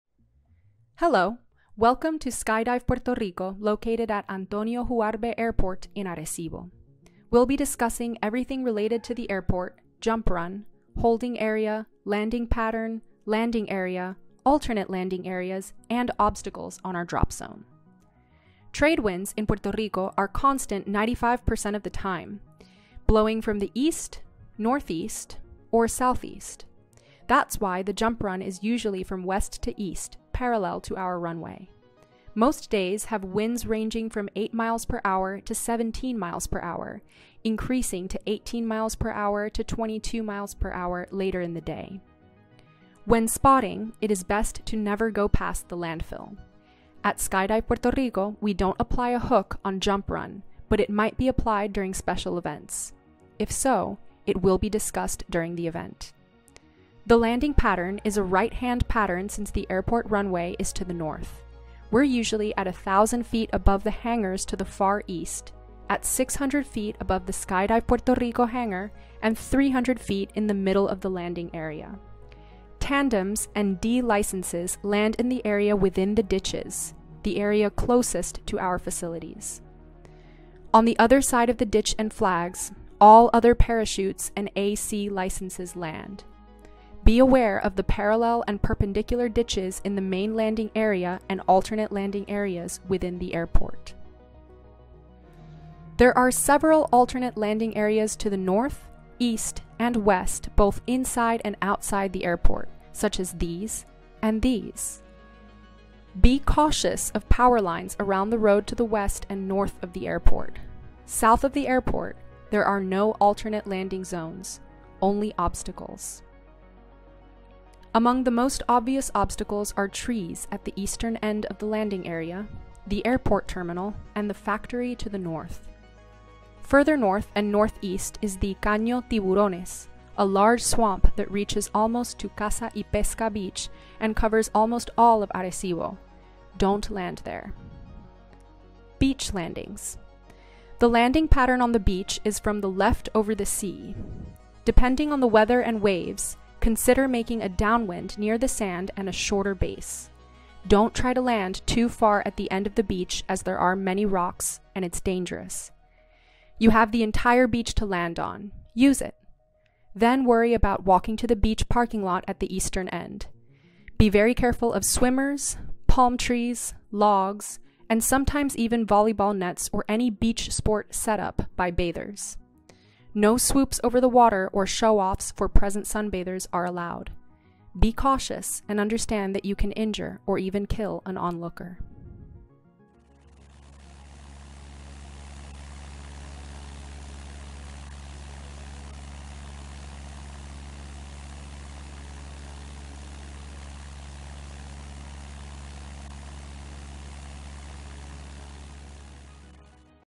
Skydive Puerto Rico DZ Brief
On this DZ Brief, we discuss everything related to the airport, jump run, holding area, landing pattern, alternate landing areas and beach landings.